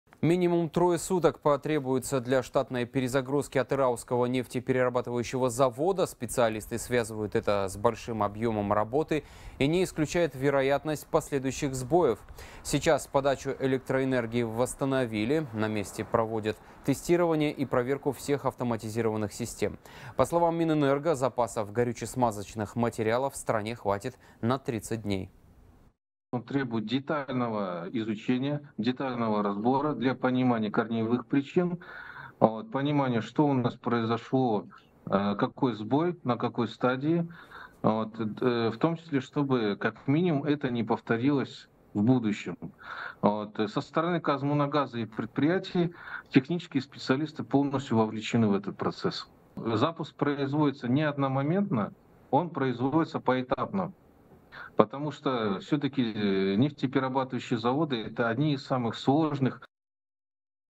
Минимум трое суток потребуется для перезагрузки Атырауского НПЗ
Минимум трое суток потребуется для штатной перезагрузки Атырауского нефтеперерабатывающего завода. Специалисты связывают это с большим объёмом работы и не исключают вероятность последующих сбоев